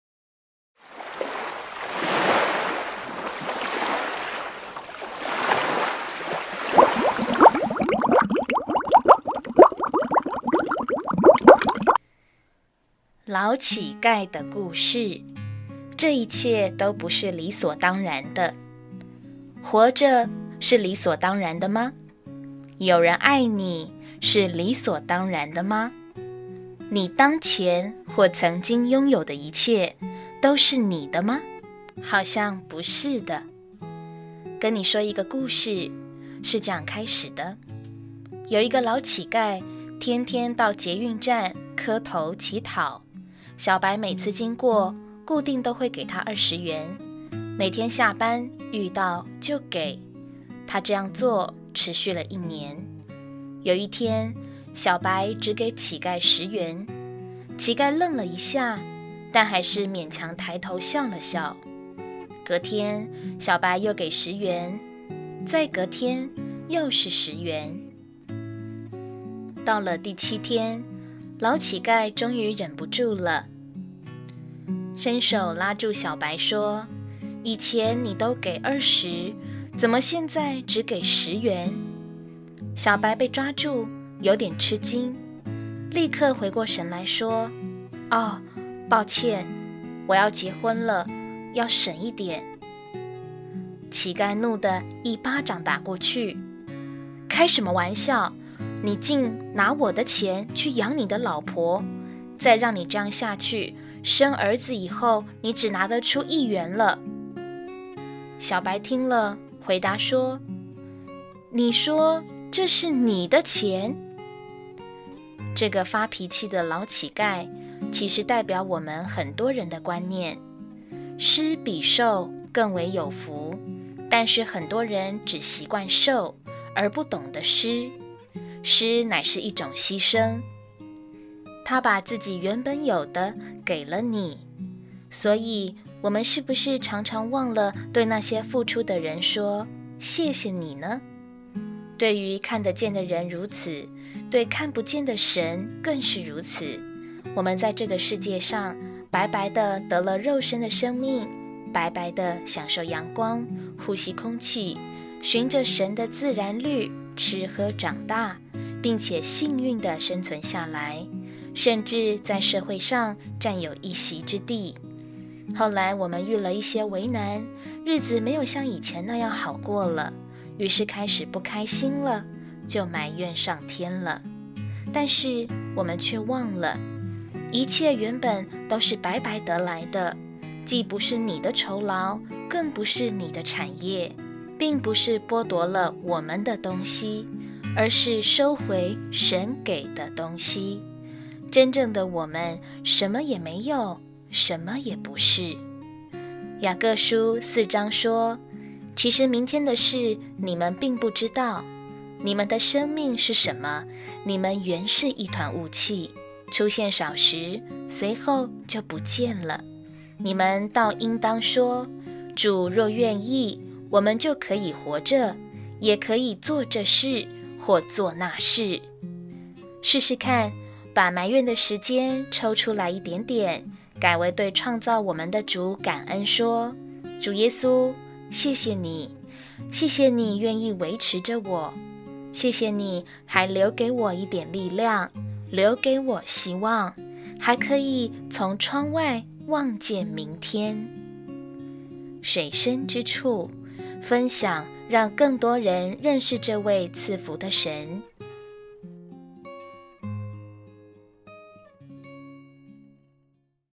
【水深聽故事】老乞丐的故事
我們太習慣別人的給予，卻忘了那並不是理所當然。
來聽聽看這個故事吧 Music by Aaron Wang